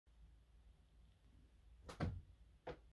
I throw my sock at the roof every day until I've changed the world: Day 1626
Sock: Yellow sock with "These early mornings are killing me" sun pattern
Roof: My Apartment